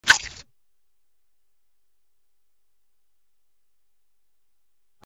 Juicy Squelch | Sound Effect
Experience the satisfyingly squishy and juicy sound of our exclusive sound effect!

This high-quality audio captures the wet and squelchy sound of something being squeezed or stepped on, perfect for enhancing your videos, games, or animations with a touch of realism.
Whether you need to depict a squashed fruit, a muddy step, or a gooey creature, this sound effect will bring your visuals to life.

Download the sample now and add a splash of authenticity to your project!
Don't miss out on this unique auditory experience!